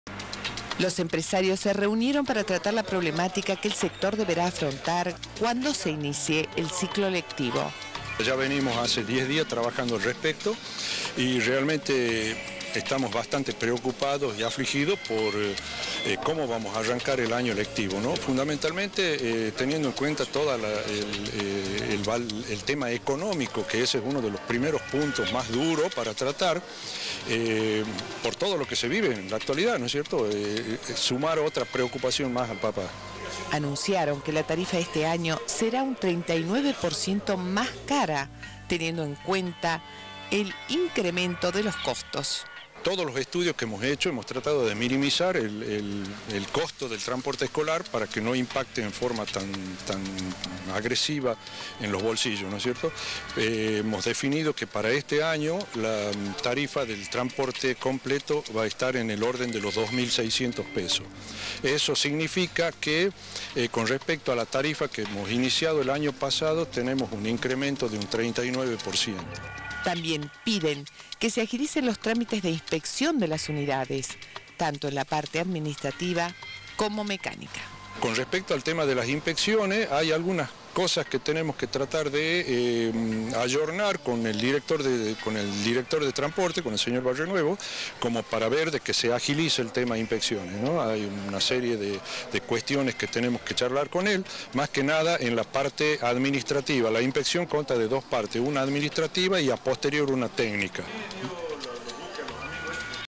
Transportistas escolares: Suben las cuotas un 39%
Preocupa a los transportistas las dificultades con que se encontraron este año, en especial, el incremento de los costos. Fijaron las tarifas un 39 por ciento más altas que el año pasado. También analizan otros temas que afectan al sector.